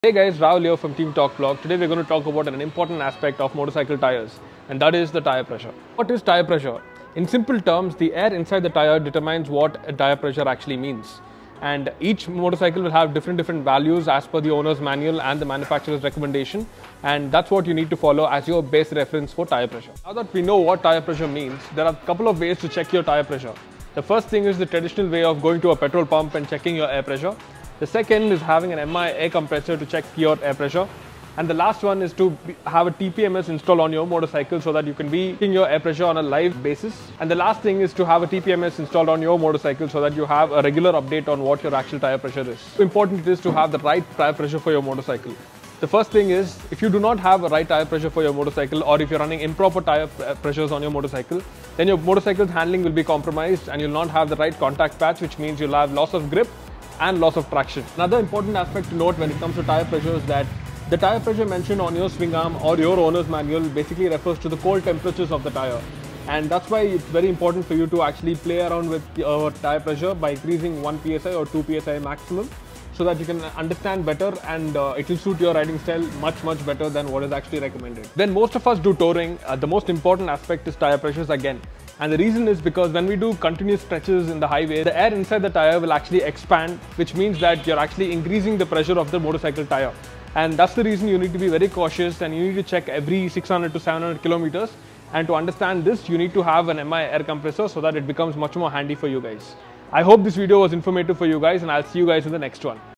Why It's Very Important To Maintain The Right Tyre Pressure? || Torque Block
To Get Your New Bike/Car Tyres Or Any Performance Parts  DM @torqueblock (Instagram)

Correct air pressure in a tyre helps to distribute the weight of your vehicle evenly across the tyre's tread pattern, so that the tyre – and the vehicle itself – is at its most stable. When a tyre is over- or under-inflated, it loses stability, negatively affecting handling, cornering and stopping.